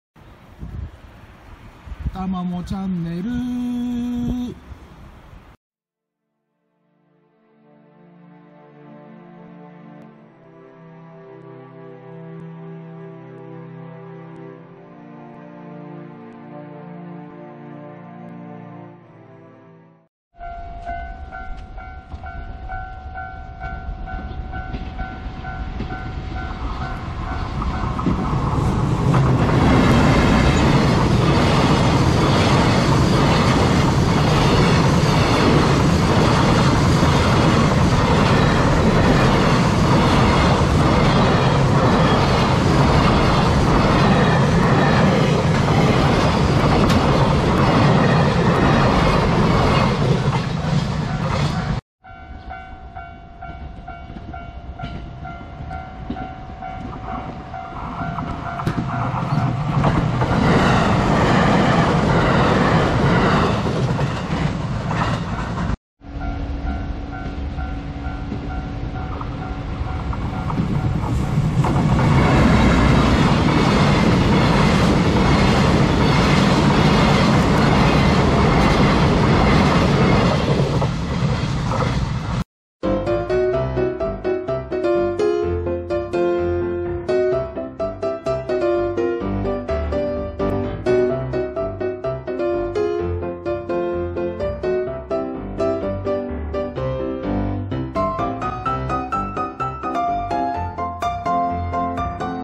岩国市の東洋紡前踏切を通過する下り8091レ日鐵チキ返空と71レと227系回送
岩国市の東洋紡前踏切を通過する貨物列車2選と227系3連の回送電車です。
8091レ　日鐵チキ（レ－ル運搬貨物列車）貨車返却　EF210－129
71レ　EF210－306

使用音源：魔王魂：YouTube オーディオライブラリー　（使用してない動画もあります。）